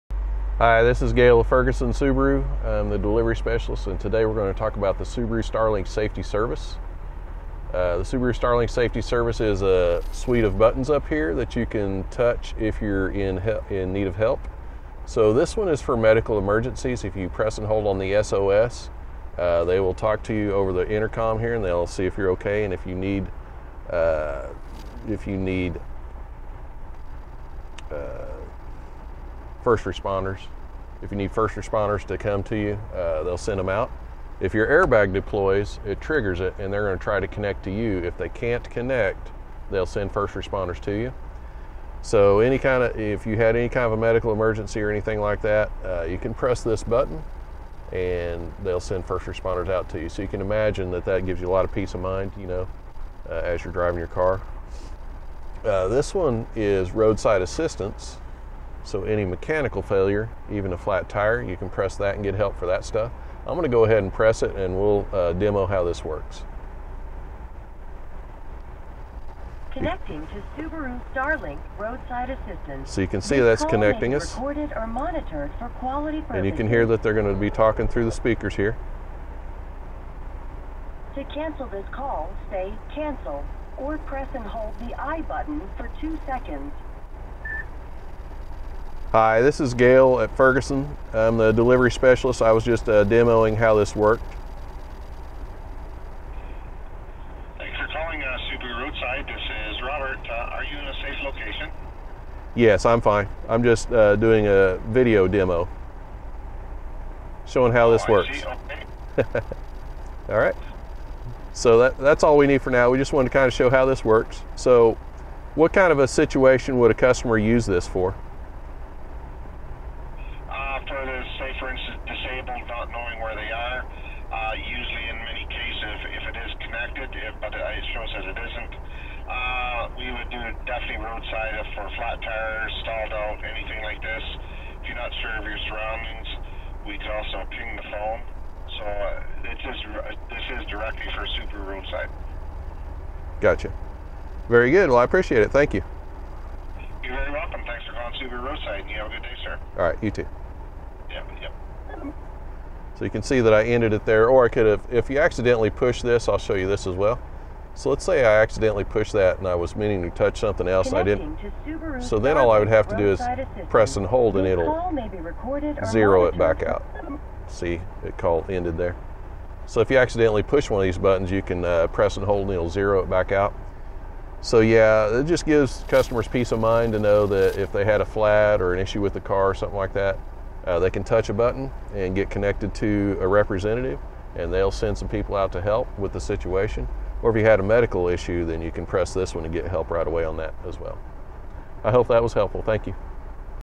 Subaru Starlink Safety Service
Subaru STARLINK Safety and Security offers services designed to help you in your time of need by keeping you and your Subaru safe whether you are in your vehicle, at your computer, or on your mobile device. These services enhance your Subaru experience with access to emergency services and convenience features. These include Automatic Collision Notification, Enhanced Roadside Assistance, Stolen Vehicle Recovery, Remote Services, Vehicle Diagnostic Alerts and more.